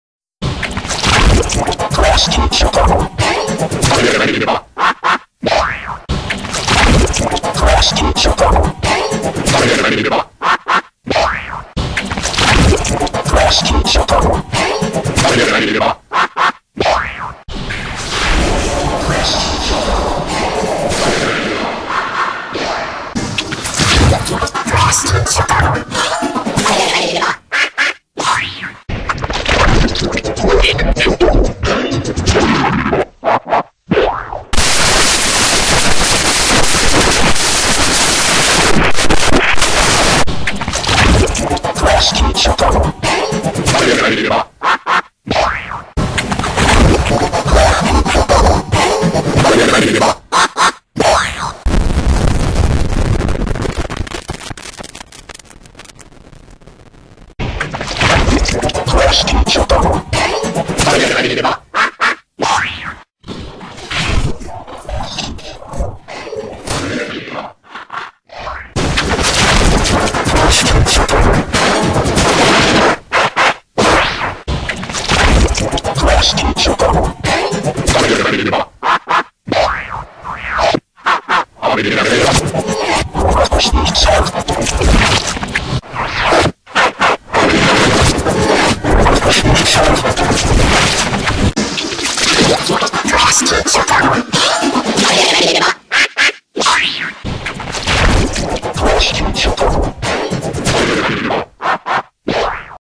(NEW EFFECT) Klasky Csupo Effects 33 in Super Weird Cylinder (Instructions in Description)
Instructions:

Apply 4 Cylinders, edit and full fade:

First Cylinder:

Center: 215
Radius: 480
Degree: 10 (Horizontal)

Second Cylinder:

Center: 450
Radius: 480
Degree: 10 (Horizontal)

Third Cylinder:

Center: 245
Radius: 480
Degree: 10 (Click Vertical)

Fourth Cylinder:

Center: 245
Radius: 480
Degree: 10 (Click Vertical again)

Hue: -60

Pitches:
100
120
125
130

Effect by GumballVideoEditor909 / GVE909